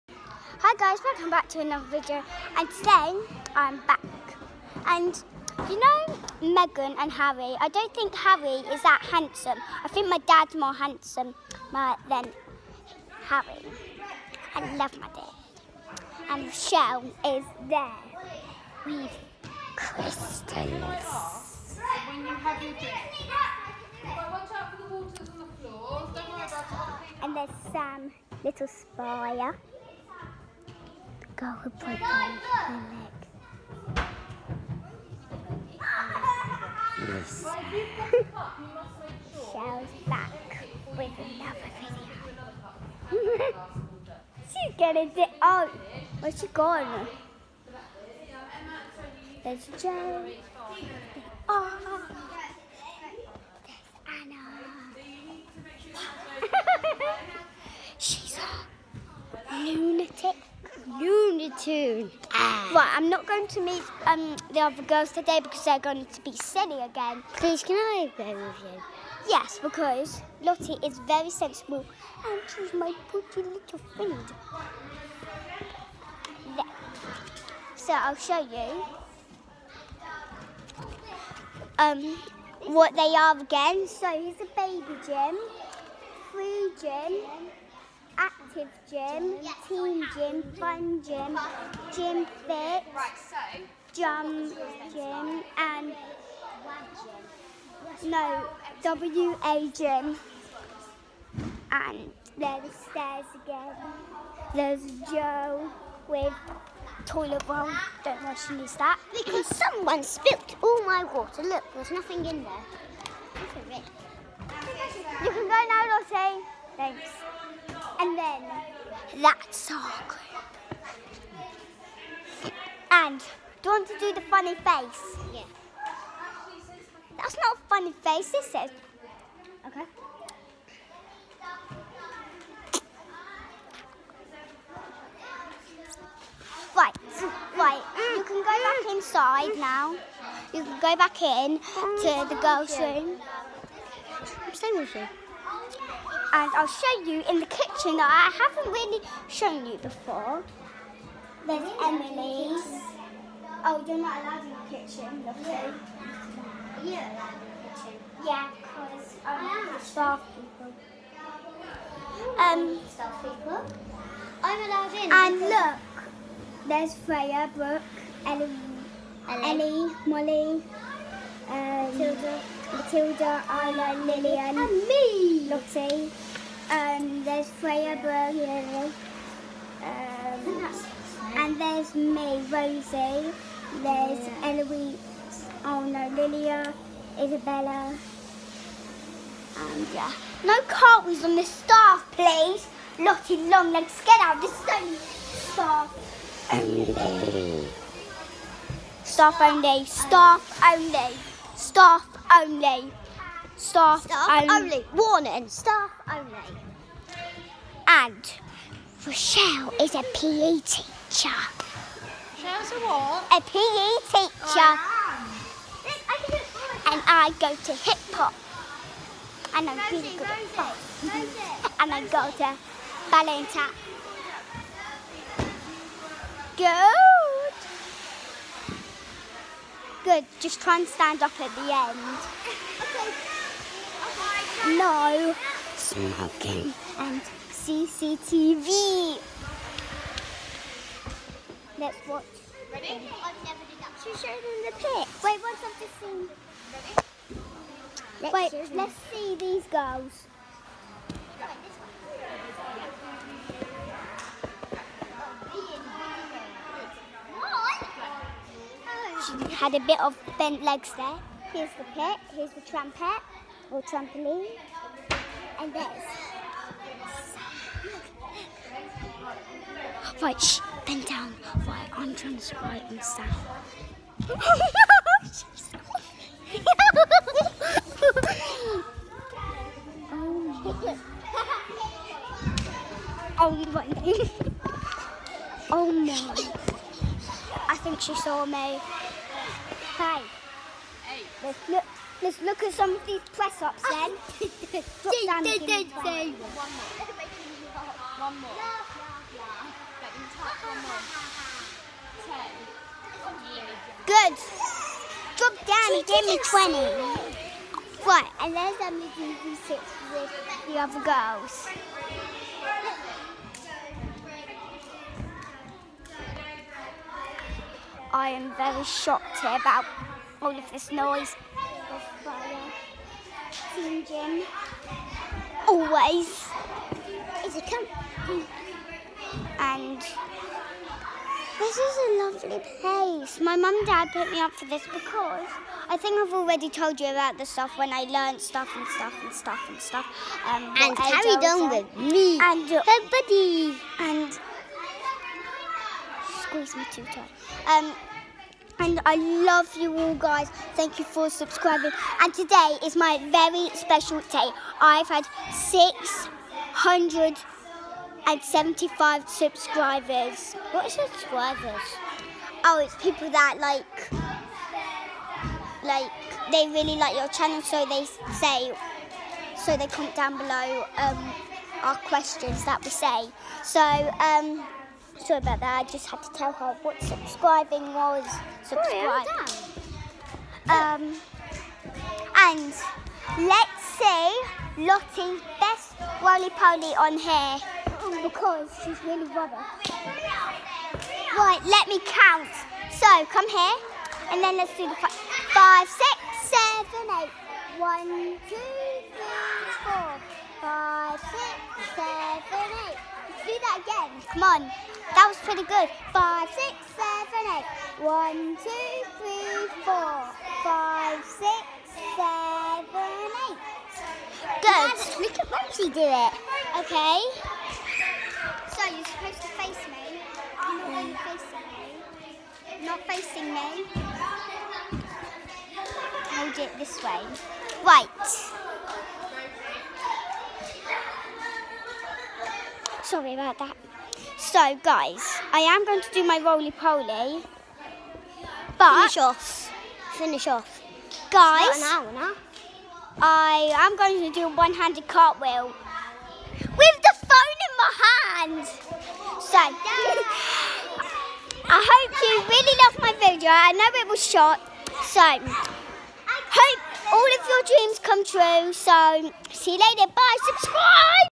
Prince Harry - Life of a PSG gymnast: Age 7.
Vlog 2 - Prince Harry

Team Sparkles gymnast Rosie Vlogs her gymnastics life. TeamGym Development Sparkles Age 6/7.